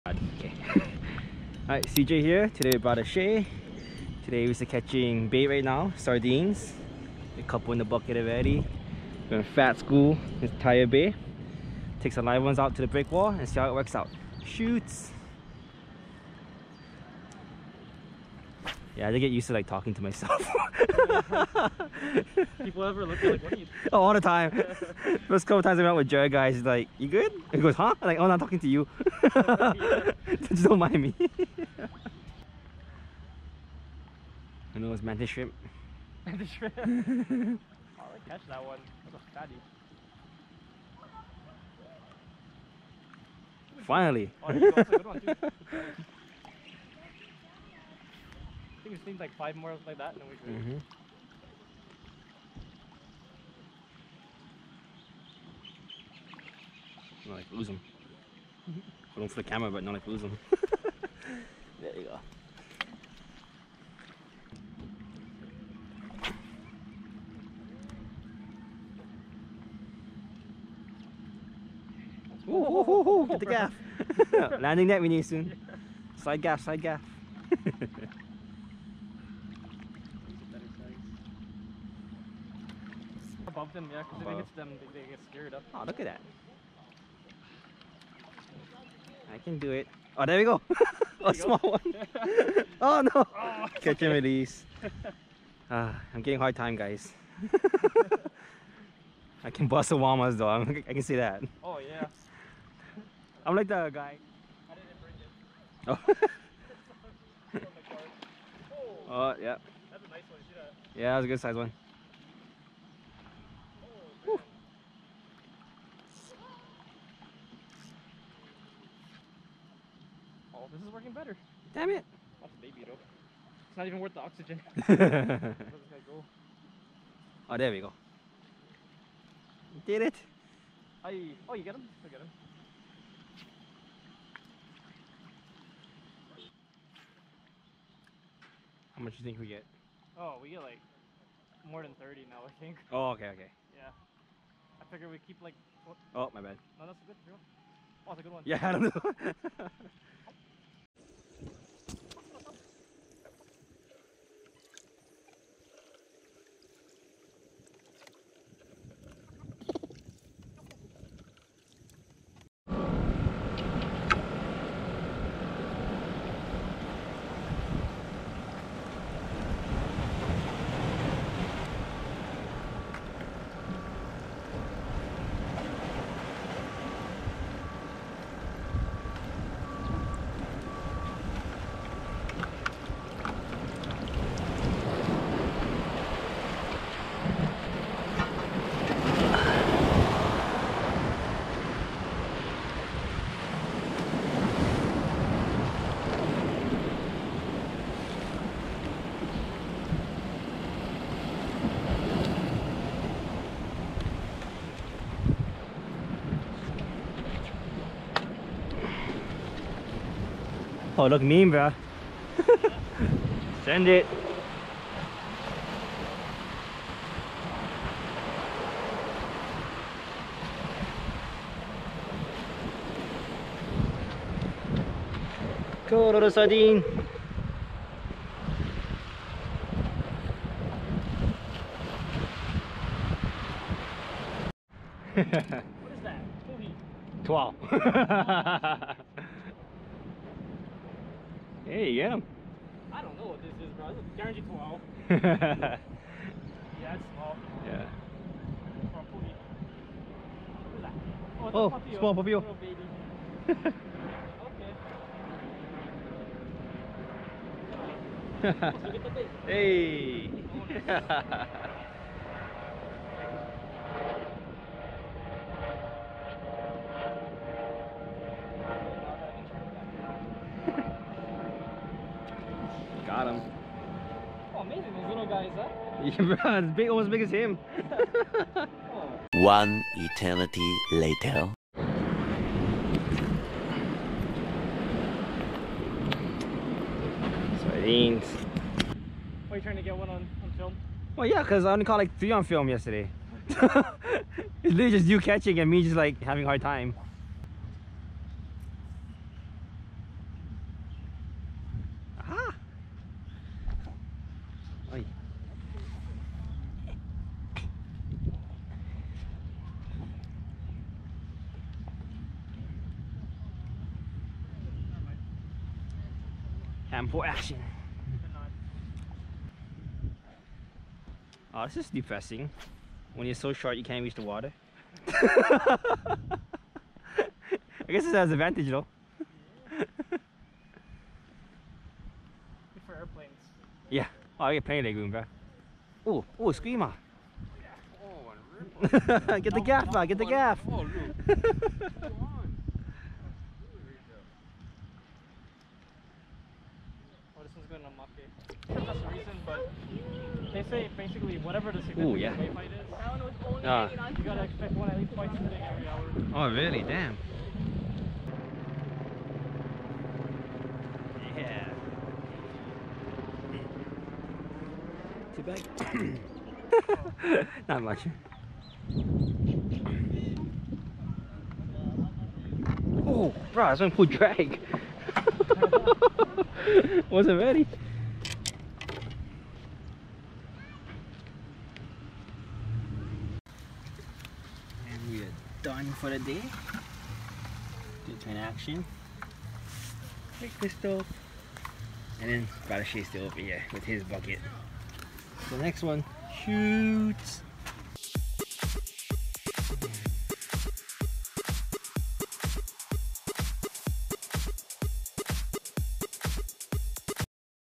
Fishing for Sardines in Hawaii!
Decided to try my hand at Sardine Fishing...  Fun type of fishing with friends, go try it out.